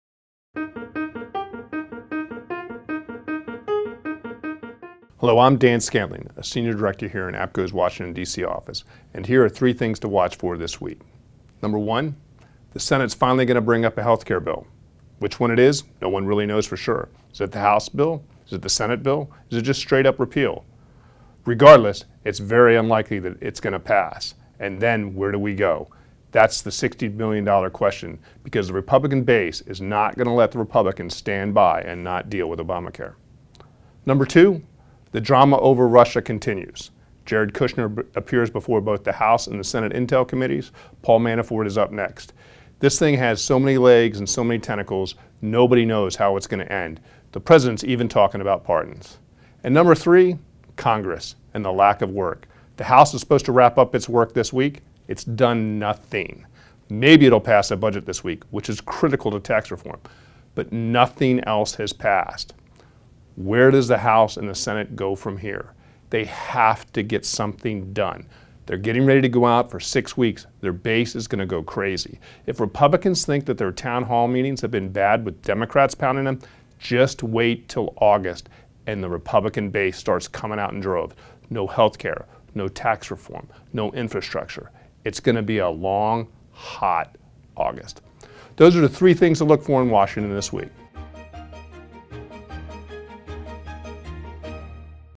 Three Things to Look for in Washington This Week – July 24, 2017
Dan Scandling, a senior director from APCO’s Washington, D.C. office, previews the week ahead and shares the three things you need to look for in DC this week.